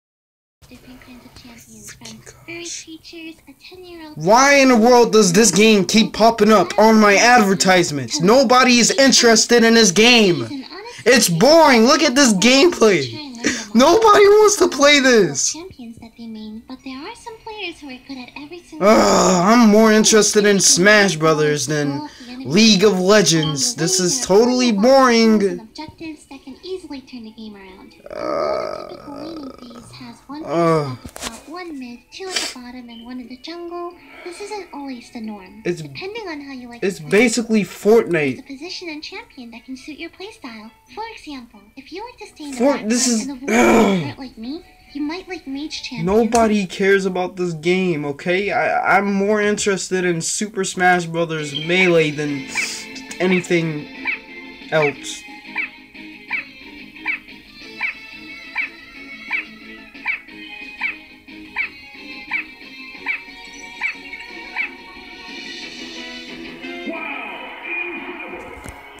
NOBODY CARES ABOUT LEAGUE OF LEGENDS STOP GIVING ME ADVERTISEMENTS
Has anybody else been getting alot of League or Legends Ads?